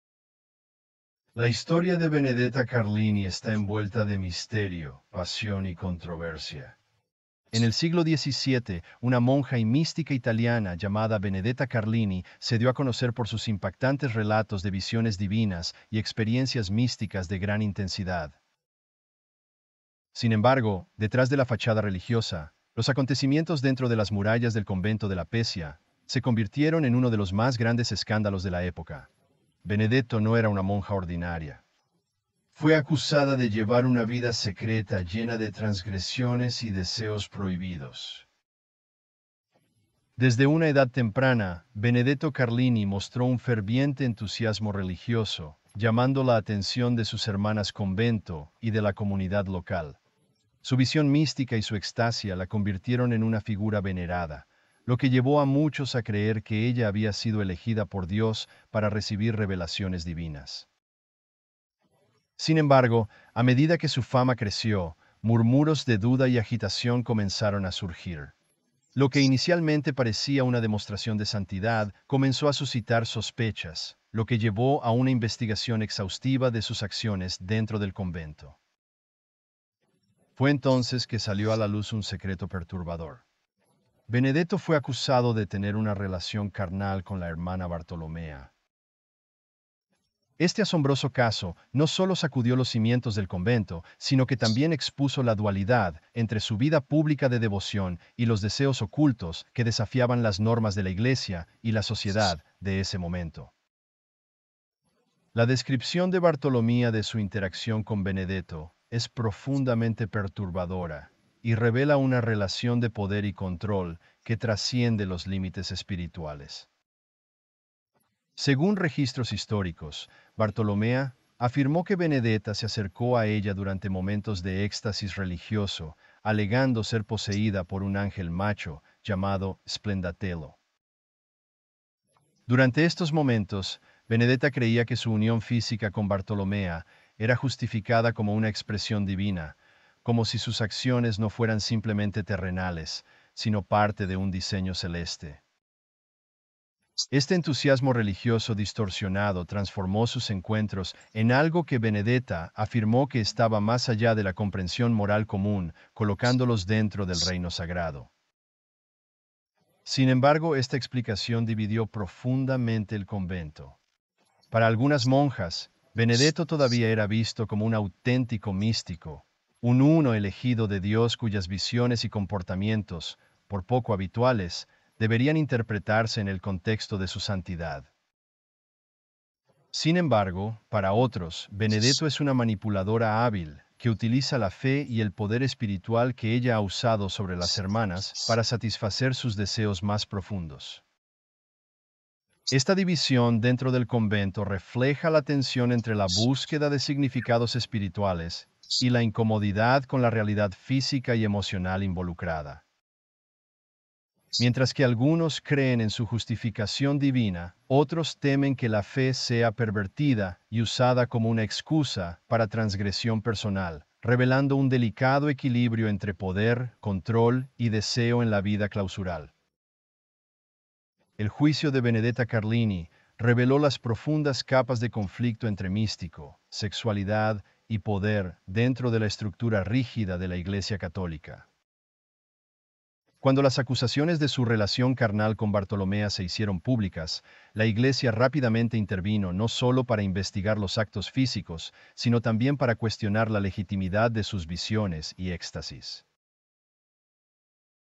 ¿Monja o hereje? Los escándalos prohibidos de Benedetta Carlini
Benedetta Carlini, monja del siglo XVII, vivió una vida rodeada de misterios, éxtasis religiosos y escándalos que sacudieron las estructuras de la Iglesia católica. Nacida en Pescia, Italia, Benedetta mostró signos de misticismo desde temprana edad, relatando visiones divinas y experimentando éxtasis que la colocaron en una posición destacada en el convento donde vivía. Se creía que había sido tocada por Dios y muchos la veían como una santa potencial. Sin embargo, detrás de su fachada espiritual se escondían una serie de hechos prohibidos que culminaron en uno de los juicios más controvertidos de la historia eclesiástica.

Con el tiempo, Benedetta fue acusada de tener una relación carnal con otra monja, Bartolomea, una joven que se convirtió en el centro de atención cuando contaba sus experiencias íntimas con Benedetta. Lo más impresionante es que Benedetta justificó estos actos afirmando estar poseída por un ángel masculino llamado Splenditello, quien la había guiado durante sus interacciones con Bartolomea. Esta explicación, que mezclaba el fervor religioso con deseos prohibidos, generó una profunda división en el seno del convento. Mientras algunas hermanas creían que Benedetta era una verdadera mística, otras sospechaban que manipulaba la fe para satisfacer sus deseos personales, algo que la Iglesia pronto decidió investigar.